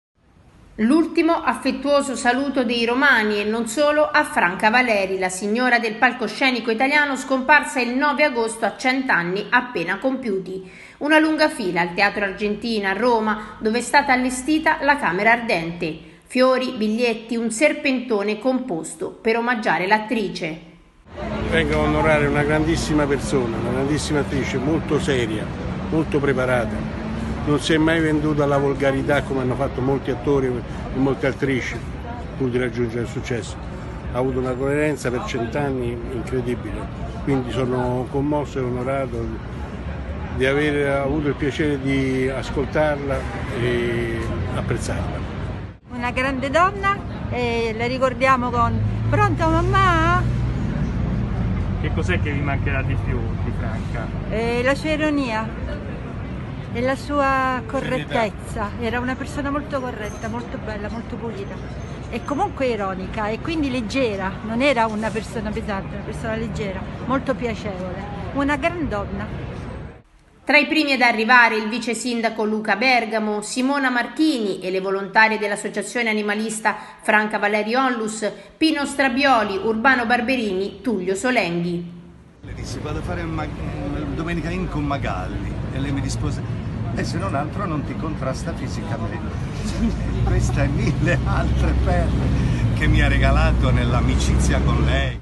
Al Teatro Argentina lunga fila per ultimo saluto a Franca Valeri
Roma, 10 ago. (askanews) - L'ultimo affettuoso saluto dei romani, e non solo, a Franca Valeri, la signora del palcoscenico italiano scomparsa il 9 agosto a 100 anni appena compiuti. Una lunga fila al Teatro Argentina, a Roma, dove è stata allestita la camera ardente.
Fiori, biglietti, un serpentone composto per omaggiare l'attrice.
"Vengo ad onorare una grandissima persona, un'attrice molto seria, molto preparata, non si è mai venduta alla volgarità come hanno fatto molti attori e attrici, ha avuto una coerenza incredibile". Ci mancherà la sua ironia, la sua correttezza. Era una persona molto corretta, molto pulita, ironica, una persona leggera, una grande donna".
Tra i primi ad arrivare, il vicesindaco Luca Bergamo, Simona Marchini - e le volontarie dell'Associazione animalista Franca Valeri Onlus, Pino Strabioli, Urbano Barberini, Tullio Solenghi.